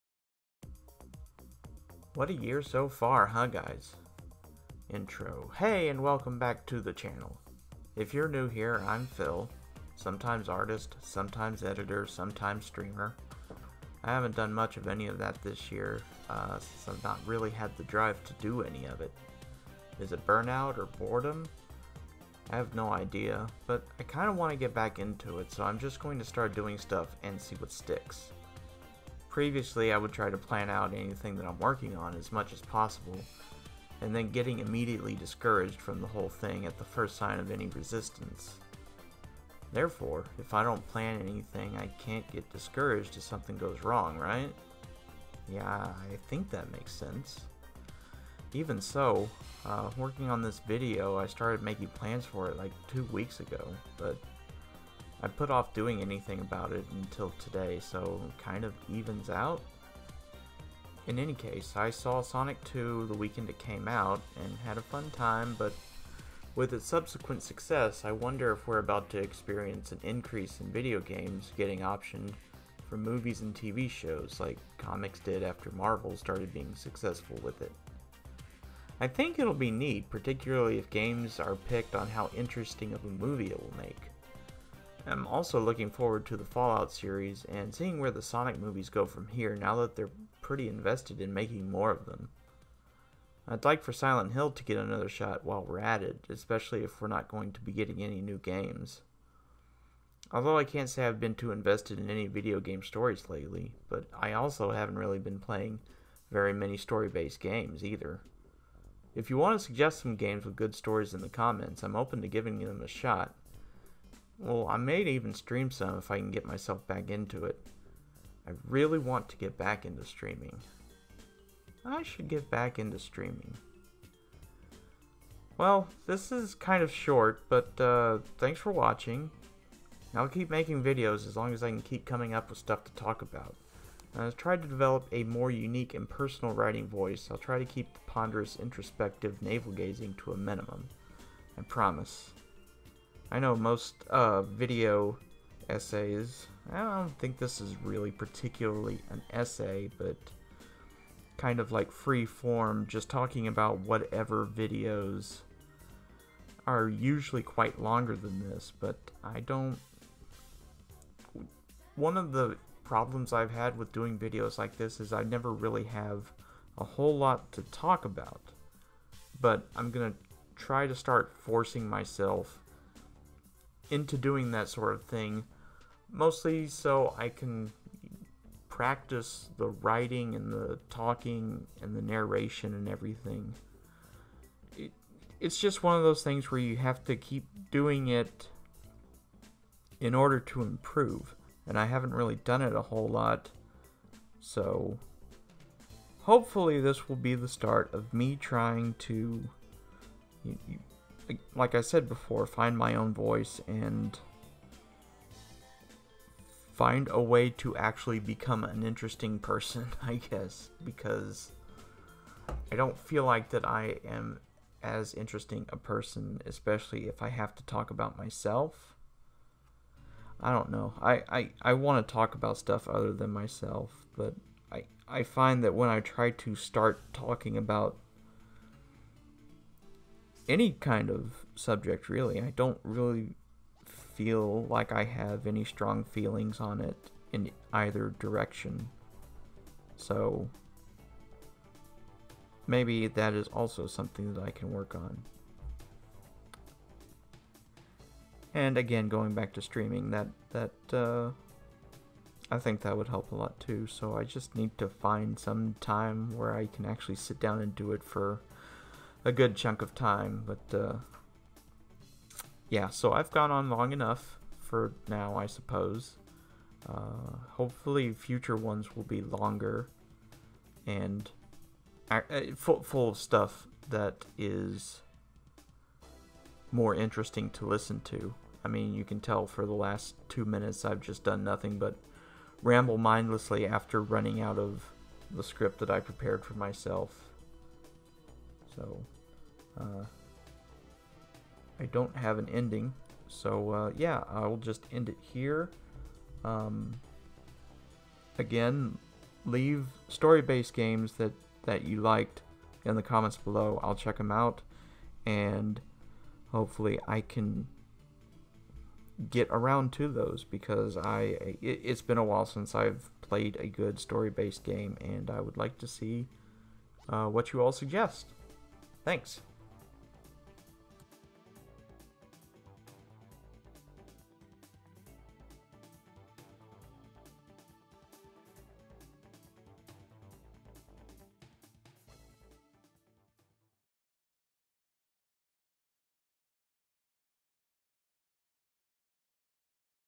An Update, Of Sorts
I'm trying to get myself back into doing something, anything, so I just prattle on for several minutes about video game movies and streaming.